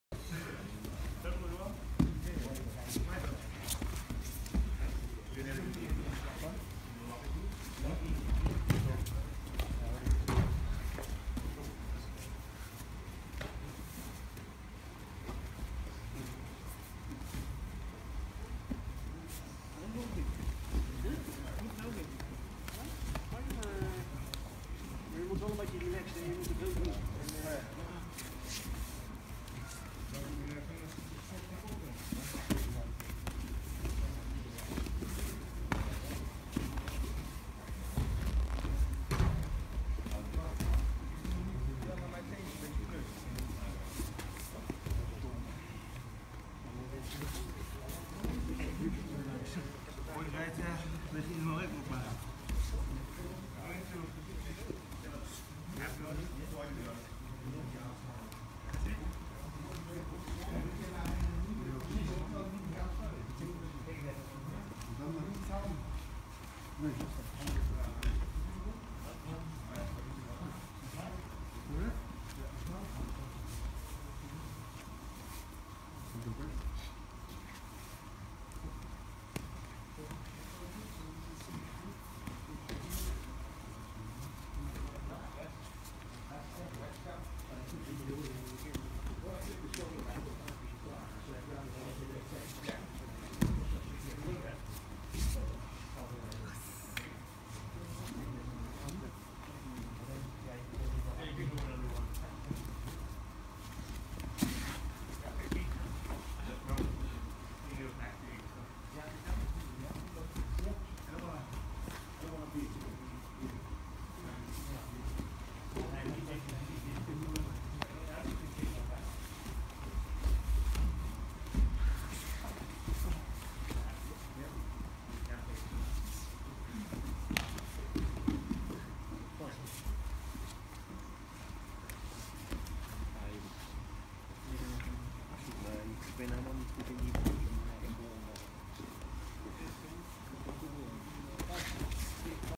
Masakazu Imanari vs Guy van Polanen
Nice to be allowed to spar with Imanari san!
I did want to play the leglock game because his is superior haha.
But if you also can't pass him and can't beat his open guard then you find yourself back in making beginners mistakes quite quickly.